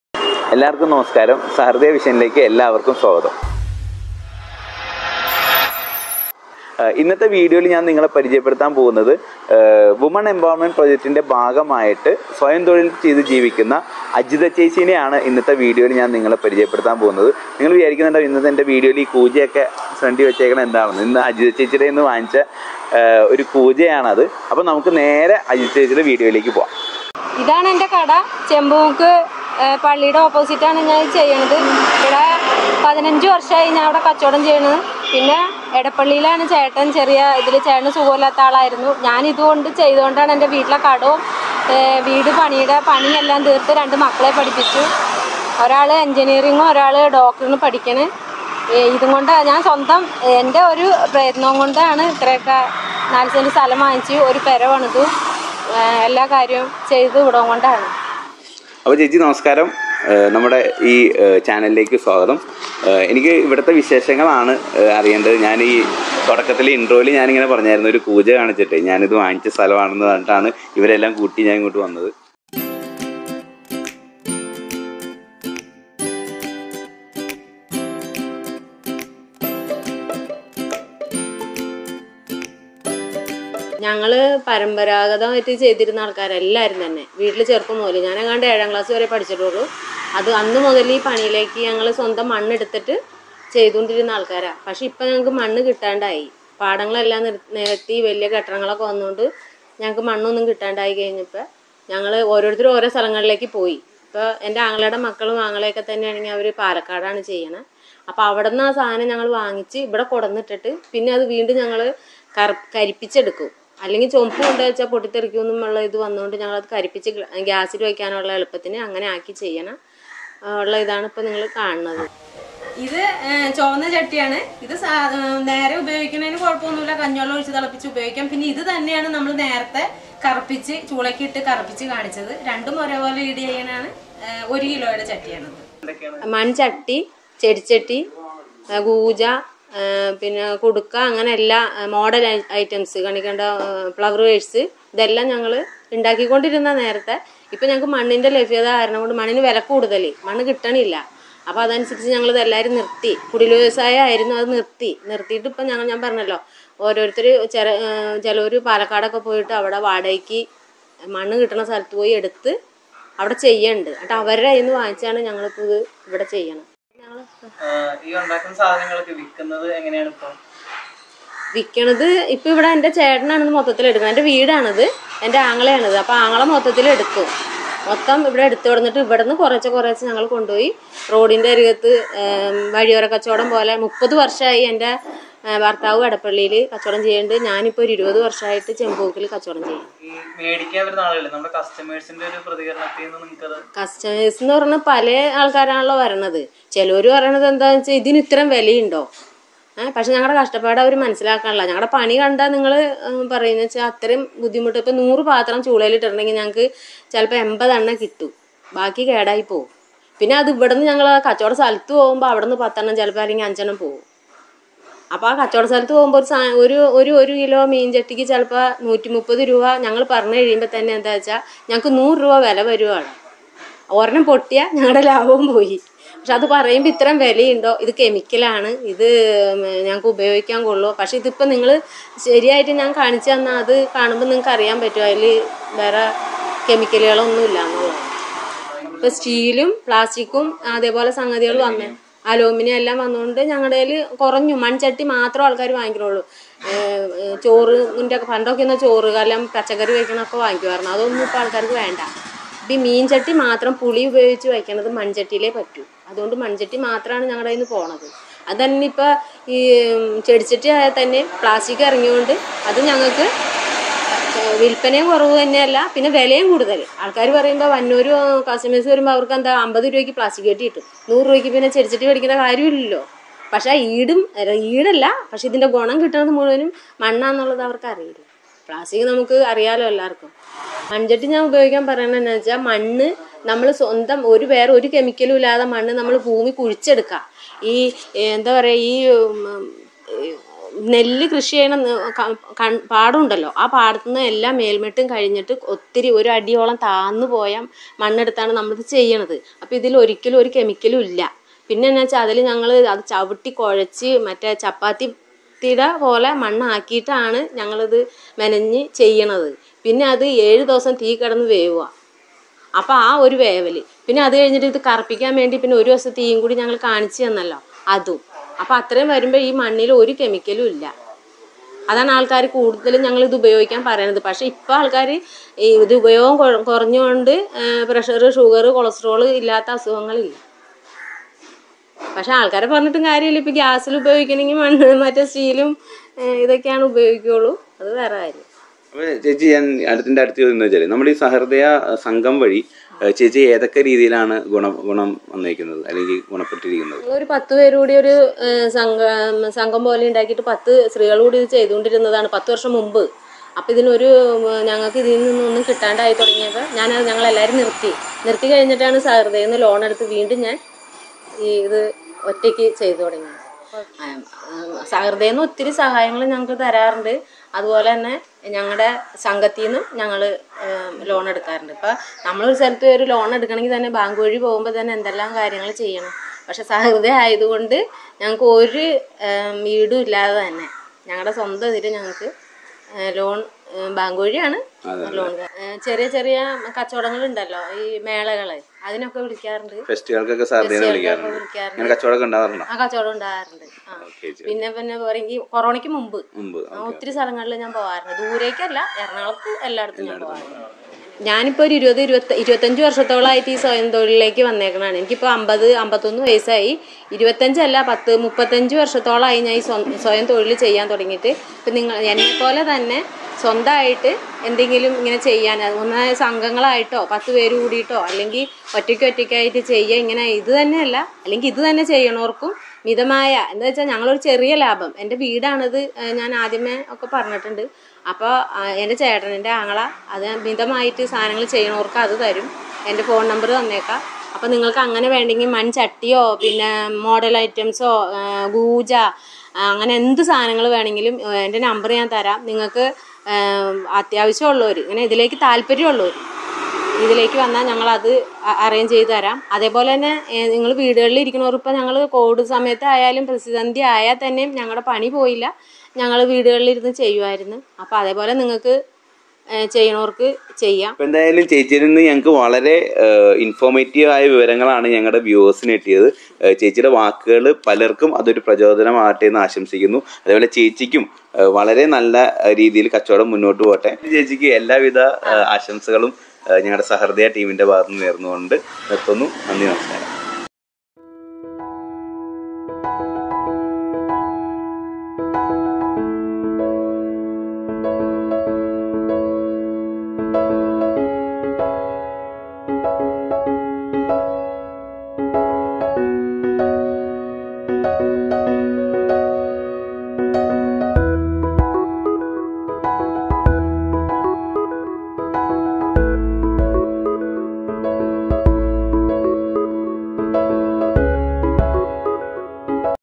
കളിമണ്ണിൽ തീർത്ത സ്വപ്‌നങ്ങൾ/ Clay Pot Making/ Making Terracotta Pots at Home
സഹൃദയ വെസ്കോ ക്രെഡിറ്റ് മുഖേന നിരവധിയാളുകൾക്കാണ് സ്വയം തൊഴിലിനായി ലോണുകൾ പ്രദാനം ചെയ്യുന്നത്. സഹൃദയ സ്വയം സഹായ സംഘങ്ങൾ വഴി ഒരുപാട് സ്ത്രീകൾക്ക് സ്വയം തൊഴിൽ ചെയ്ത് വരുമാനം കണ്ടെത്തുവാനും, സമൂഹത്തിൽ മുൻനിരയിലേക്ക് കടന്നുവരുവാനും സാധിച്ചിട്ടുണ്ട്. പൊതുസമൂഹത്തിന് തന്നെ മാതൃകയായി പ്രവർത്തിക്കുന്ന വനിതാരത്നങ്ങളെയാണ് സഹൃദയ വിഷനിലൂടെ നിങ്ങൾക്ക് മുന്നിൽ പരിചയപ്പെടുത്തുന്നത്. പരമ്പരാഗതരീതിയിൽ കളിമൺപാത്രങ്ങൾ നിർമ്മിച്ചു അവ വിറ്റു ജീവിക്കുന്ന, കുടുംബത്തിലെ അംഗമായ അജിത ചേച്ചി ഇന്നും തന്റെ പ്രവർത്തനങ്ങളിൽ കർമ്മനിരതയായി മുന്നോട്ടുപോവുകയാണ്. കോവിഡ് കാലം കച്ചവടത്തെ പ്രതികൂലമായി ബാധിച്ചുവെങ്കിലും മൺപാത്രങ്ങൾ വിറ്റാണ് തന്റെ വീട് പണിതതെന്നും,മക്കളെ പഠിപ്പിച്ചതെന്നും അജിത ചേച്ചി അടിവരയിട്ടു പറയുന്നു. കളിമൺ പാത്രങ്ങൾ, ചെടിച്ചട്ടികൾ, അലങ്കാര വസ്തുക്കൾ, കളിമൺ പ്രതിമകൾ, ഓണത്തപ്പൻ എന്നിവ നിങ്ങൾക്ക് മേടിക്കുന്നതിന് അജിത ചേച്ചിയുമായി ബന്ധപ്പെടാവുന്നതാണ്.

Main Shop Location : Puthusserymugal, Kangarappady